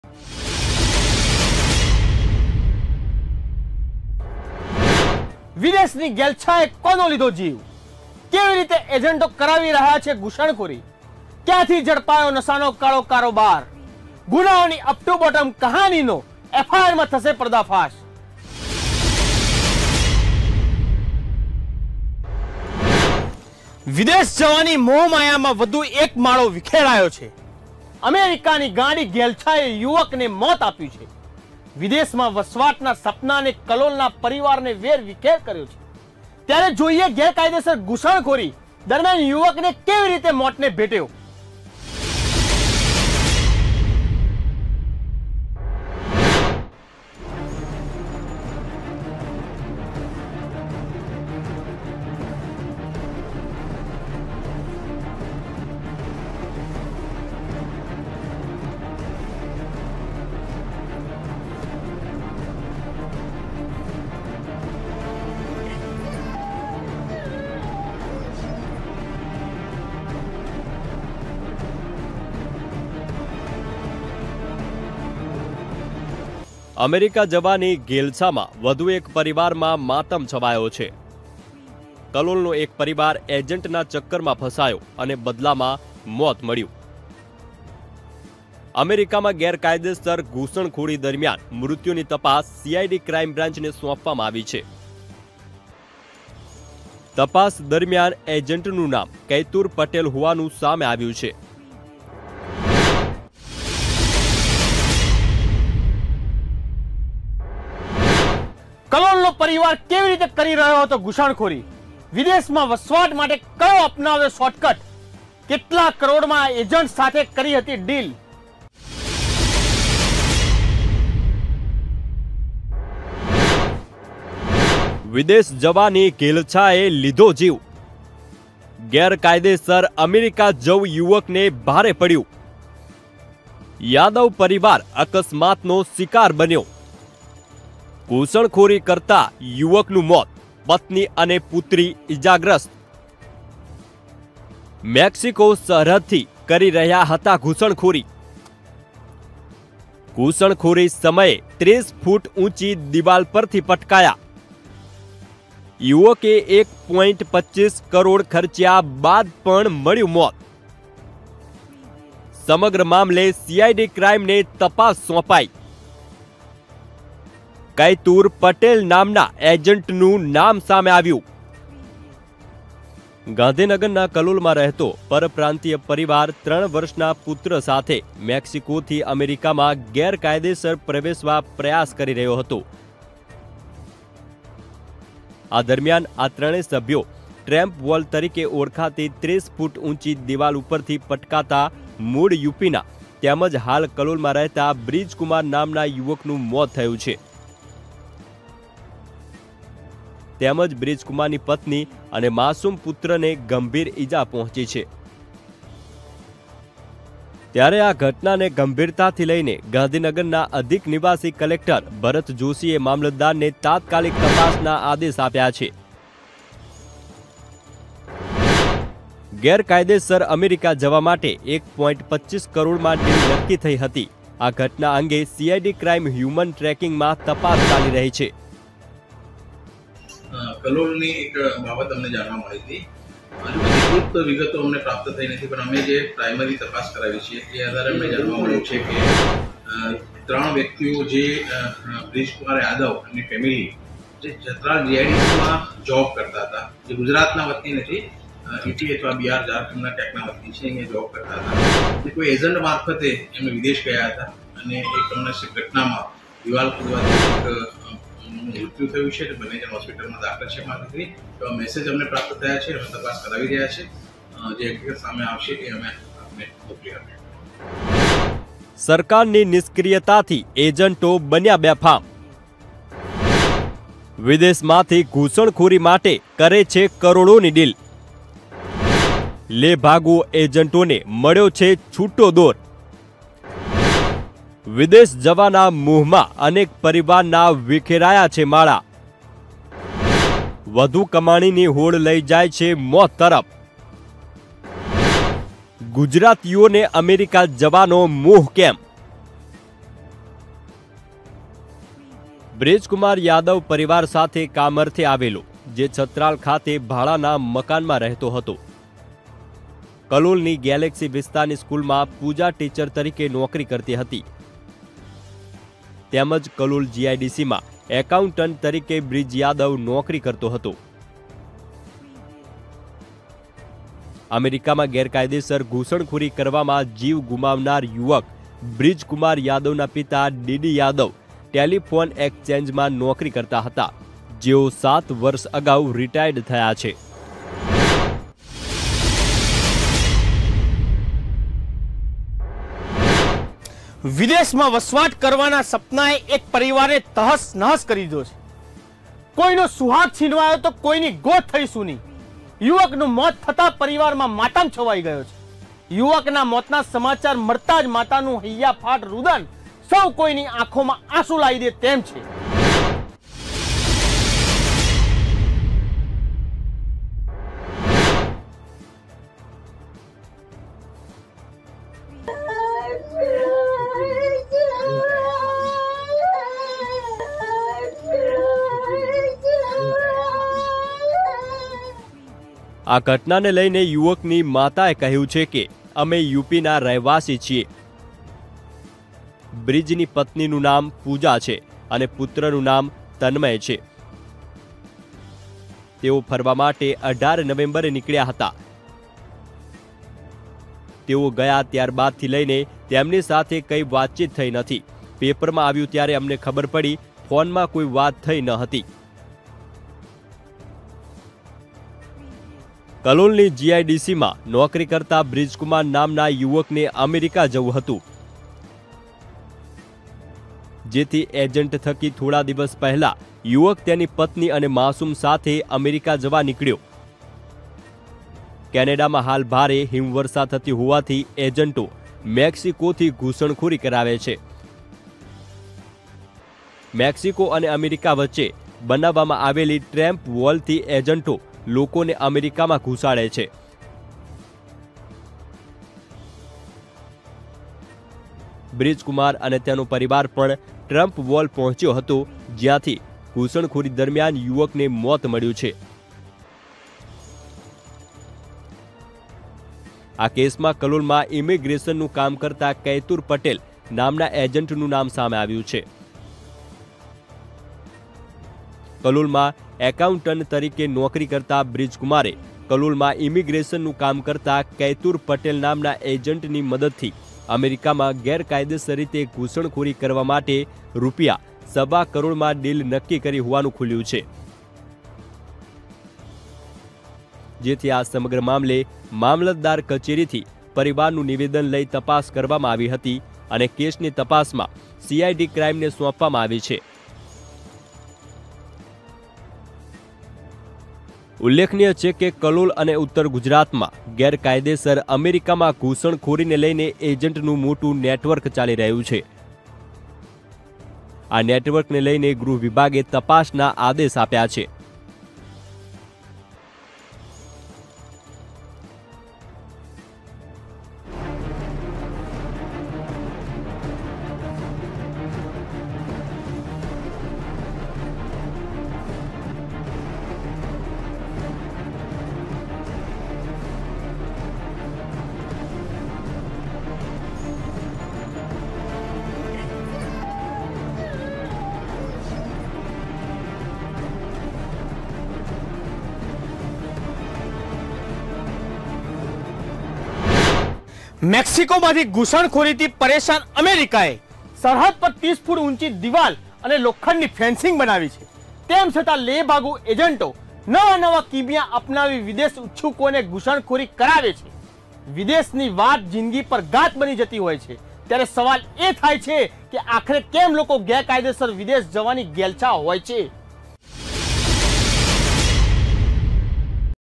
Young man dies after jumping off Us Trump Wall | અમેરિકાની ટ્રમ્પ વૉલથી કૂદવા જતાં યુવકનું મોત
પુત્ર-પત્ની ગંભીર હાલતમાં સારવાર હેઠળ
સમગ્ર મામલે CID ક્રાઈમને તપાસ સોંપાઈ
કૈતુર પટેલ નામના એજન્ટનું નામ સામે આવ્યું
એજન્ટ ભૂગર્ભમાં ઉતરી જતા પોલીસે શોધખોળ શરૂ કરી
ઘૂસણખોરીમાં વધુ એક પરિવાર વિખેરાયો
અમેરિકામાં જવા માટે 1.25 કરોડમાં ડીલ
કલોલના યુવકની એજન્ટ સાથે 1.25 કરોડમાં ડીલ